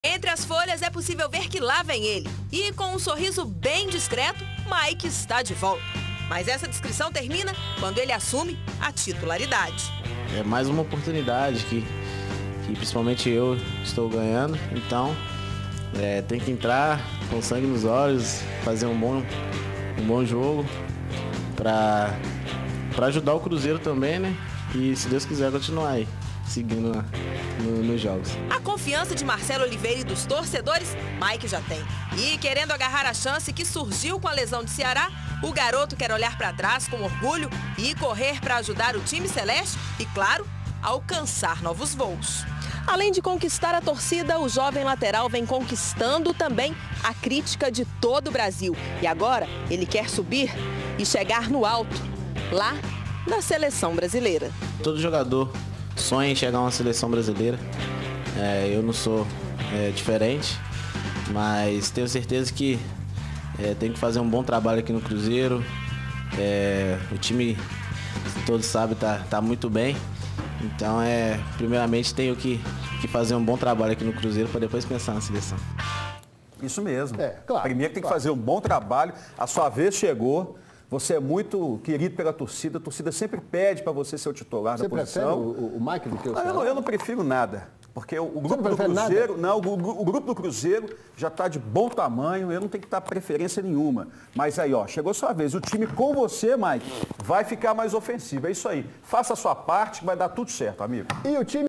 Mayke terá nova oportunidade contra Botafogo no sábado
O lateral-direito Ceará está lesionado, fato que dá oportunidade para Mayke atuar mais uma vez. O jovem lateral tem conquistado a crítica de todo o país e já sonha com Seleção Brasileira.